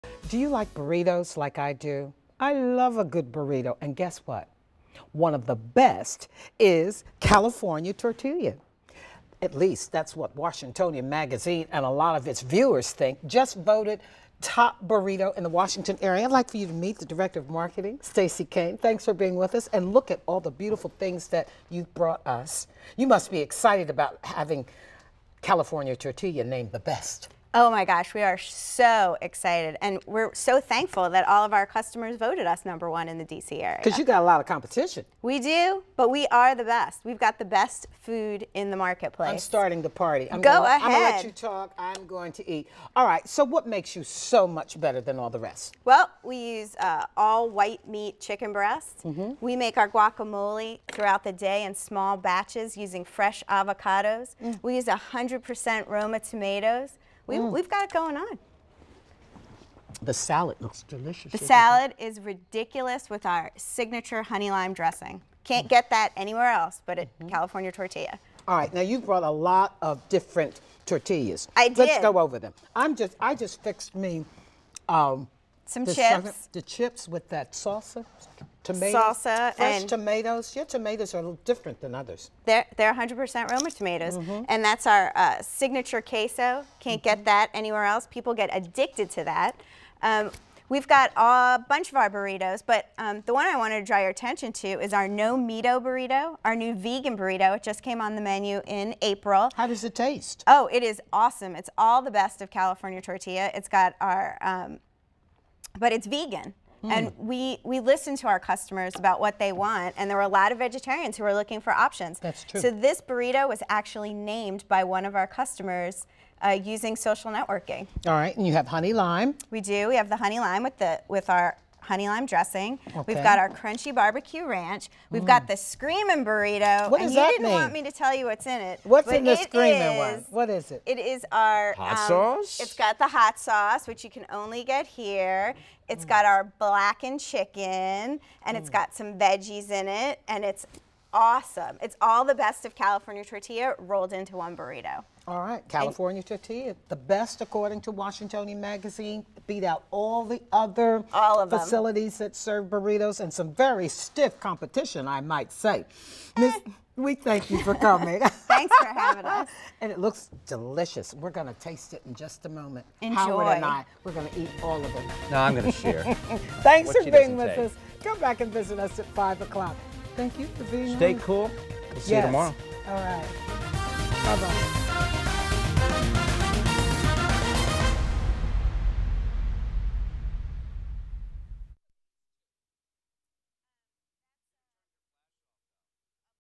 FOOD SEG WUSA.mov
CalTort's Director of Marketing Stacey Kane stopped by WUSA-9 to give J.C. Hayward the dish on what makes California Tortilla the freshest and darn tastiest Mexican Food around.  From the high quality ingredients to CalTort's devotion to its loyal customers, we take viewers beyond our burritos to show the enthusiasm and painstaking attention to detail behind every selection on California Tortilla's menu.